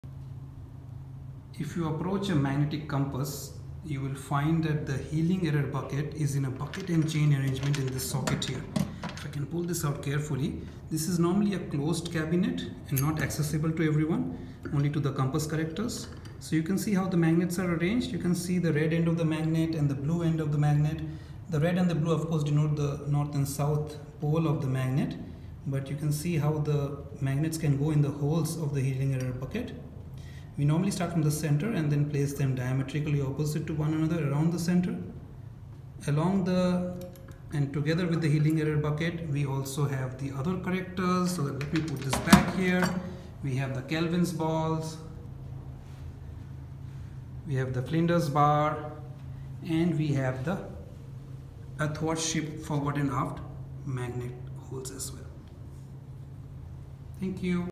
Magnetic Compass correctors - Heeling error bucket, Flinder's Bar, Kelvin's Balls
In this video, I provide a snapshot of the different hard iron and soft iron magnetic compass correctors found on ship installations.
What you see in this video:
Magnetic Compass Binnacle
Kelvin's Balls or Soft iron spheres
Heeling error bucket
Athwartship and Longitudinal compass correctors